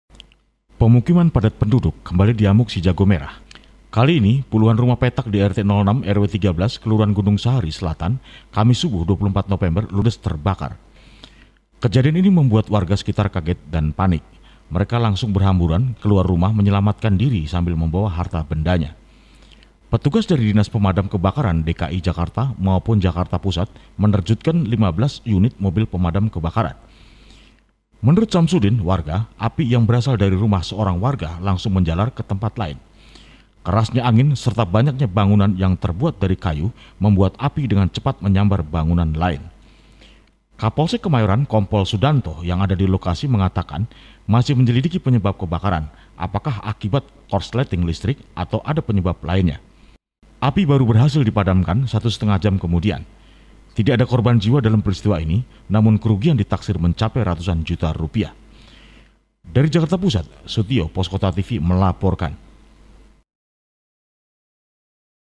Permukiman Padat di Kemayoran Terbakar
KEMAYORAN (Pos Kota) -- Pemukiman padat penduduk kembali diamuk si jago merah. Kali ini, puluhan rumah petak di RT 06/013 Kelurahan Gunung Sahari Selatan ludes terbakar, Kamis (24/11) subuh.